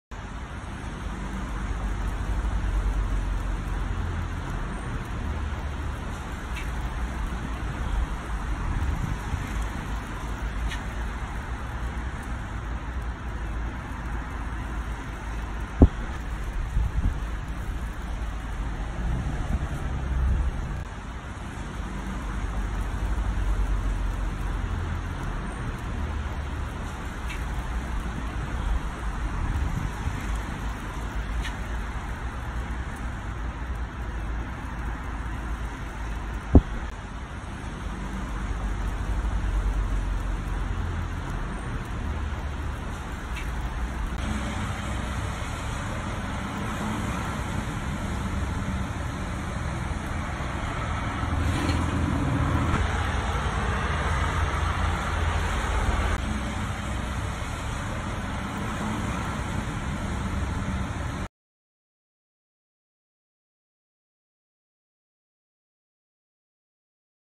Snow in Waterford | Feb 2020 | Explore Ireland | Places to Visit | Snowfall Part 2 | Attractions
Splendid Snowfall captured in Waterford, Ireland on 11-Feb-2020.